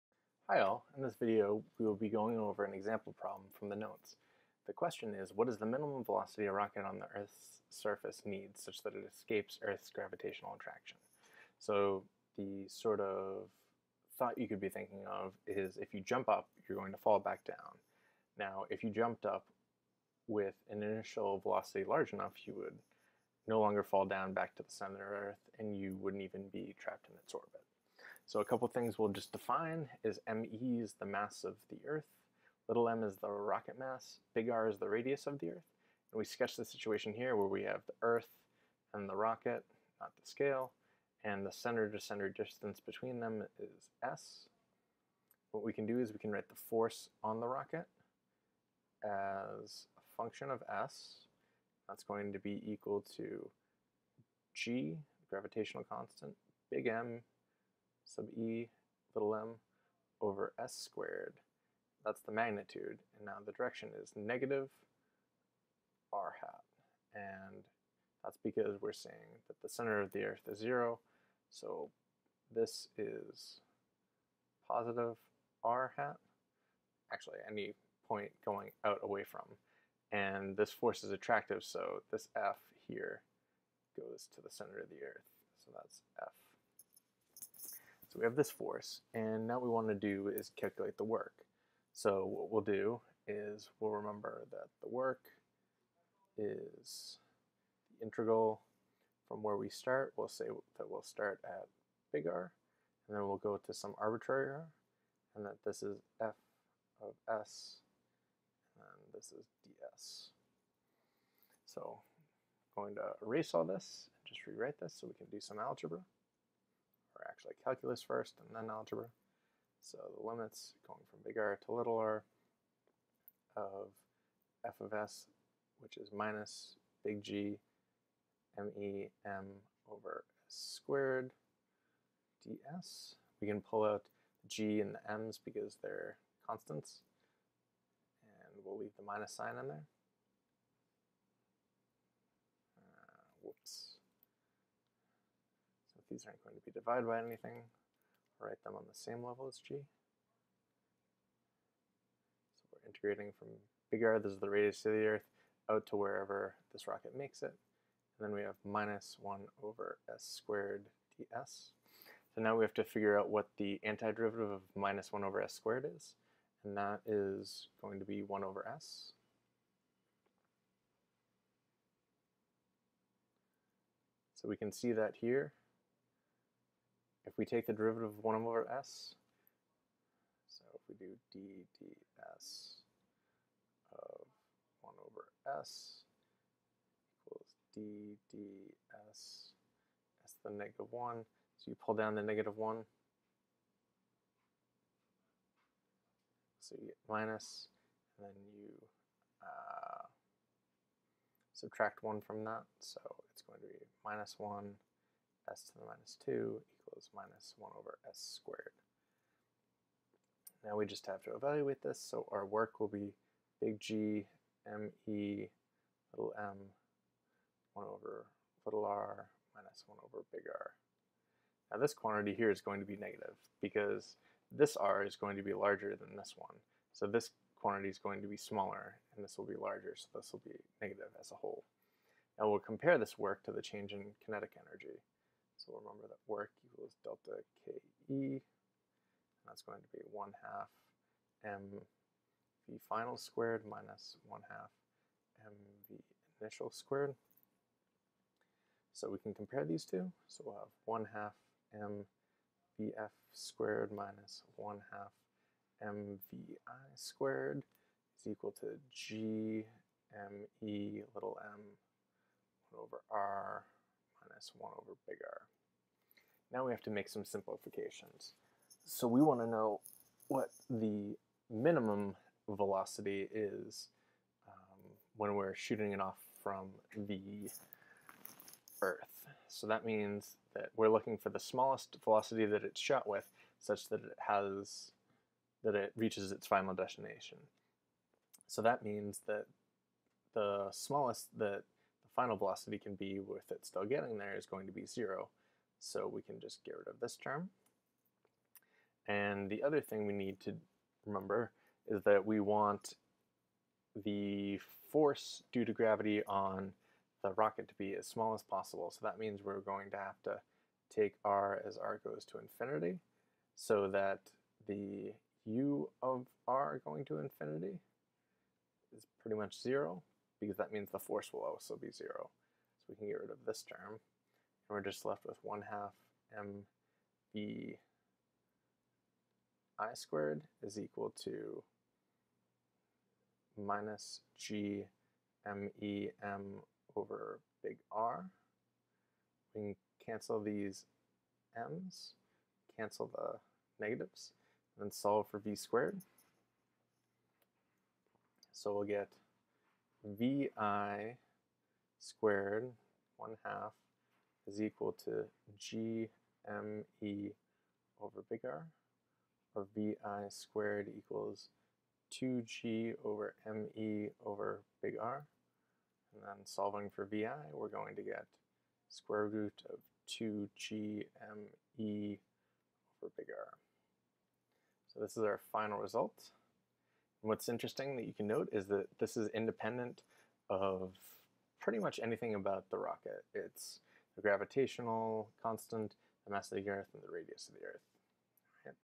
Escape Velocity
This video solves for the escape velocity of a rocket shot from the surface of the earth.